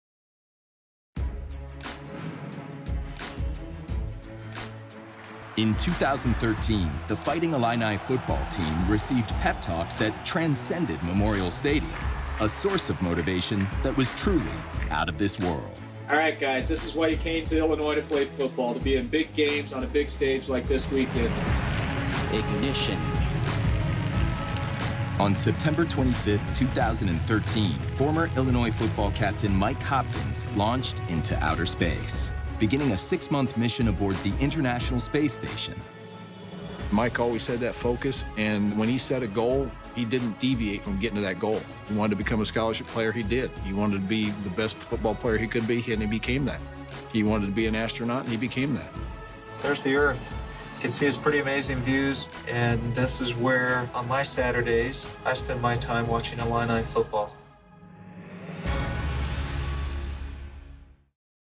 2014 LiveBIG: Floating Illini (Illinois)
Former Illinois football captain Mike Hopkins inspires the Fighting Illini community... from millions of miles away. Hopkins, an astronaut, spoke with Illinois students from the International Space Station.